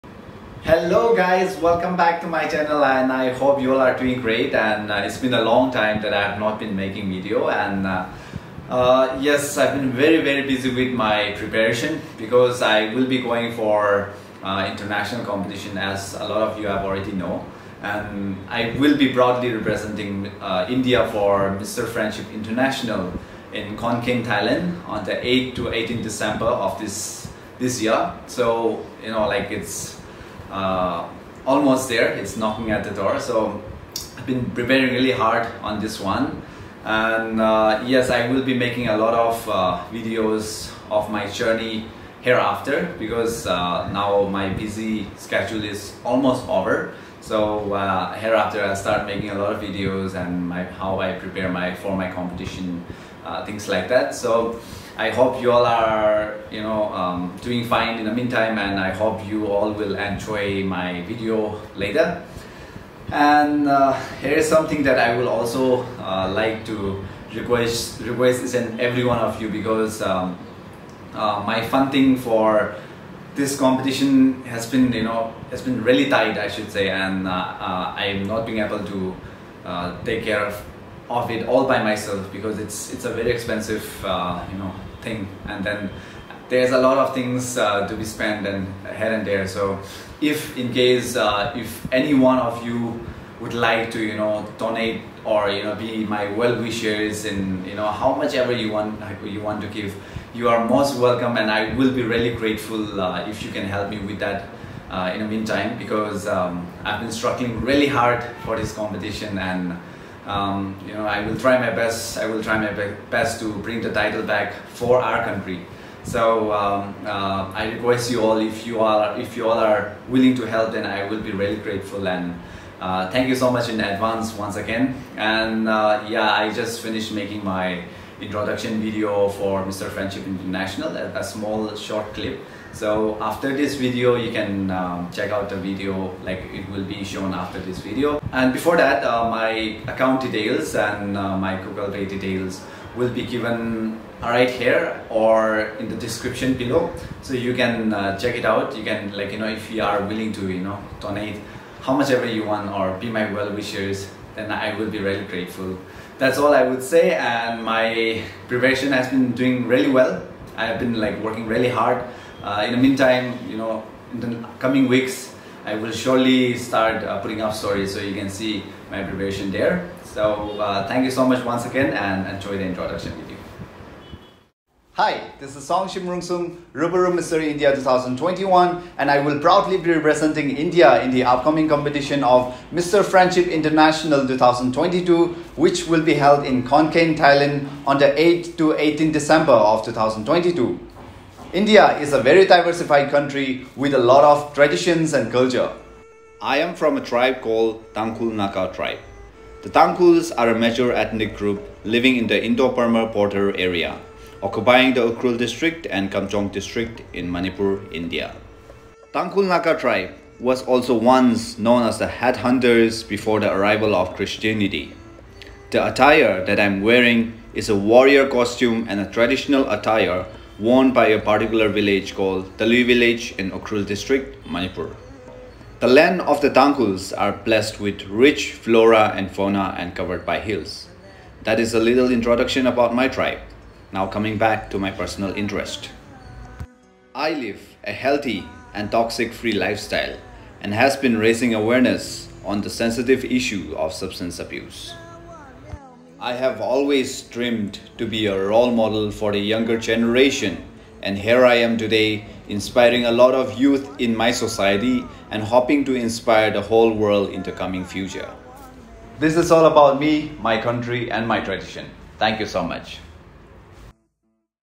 Appeal to my audience for my upcoming competition Mister Friendship International
My bank account details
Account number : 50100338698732
Account holder : Songashim Rungsung
IFSC : HDFC0000076
Branch : MG Road, Bangalore

My Google pay id: songashimrungsung@okicici